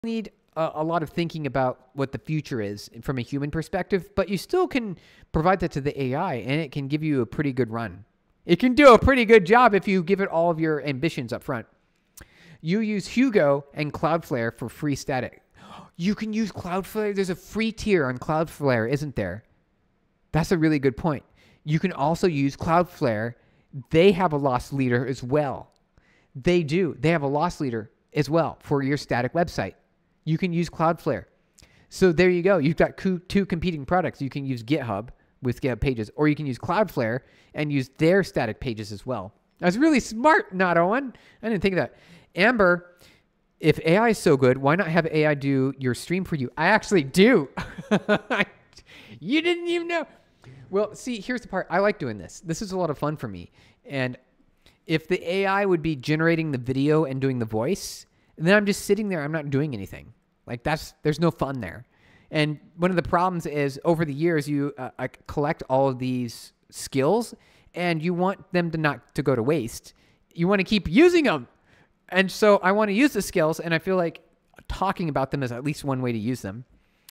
Free Static Website Hosting: Cloudflare vs. GitHub Pages
We need to think a lot about what the future looks like from a human point of view, but you can still give your ideas and goals to AI and it will do a pretty good job. If you lay out your ambitions, AI can handle a bunch of things for you. For example, you can use Hugo and Cloudflare for free static sites.

Yes, Cloudflare has a free tier, just like GitHub Pages, so you can go with either one to host your static website without paying. Both of these services are good picks and having options is always helpful. Also, about AI running a stream, I already do that a bit, but I like being a part of the process.

Making videos, doing the voice, and sharing what I know feels good because it uses the skills I have learned over the years, and I do not want them to just sit and get wasted. Talking about the things I know is one way to keep using those skills and stay involved.